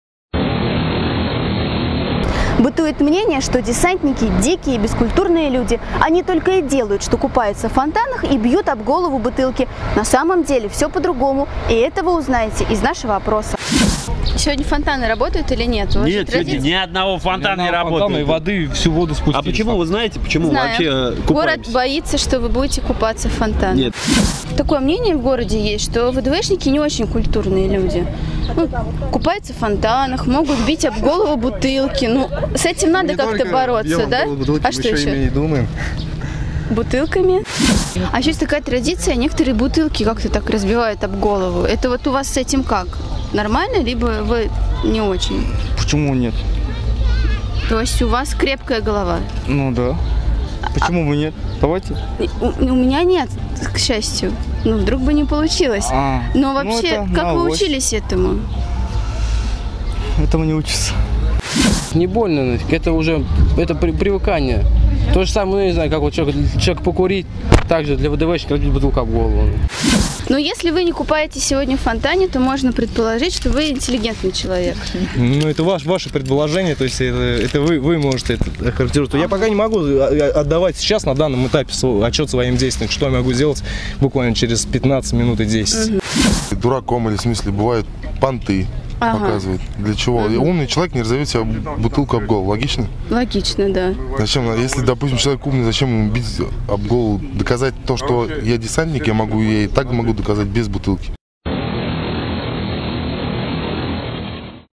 Никто кроме них
Редакция The Saratov Rooms решила опровергнуть миф о том, что все десантники бьют об голову бутылки.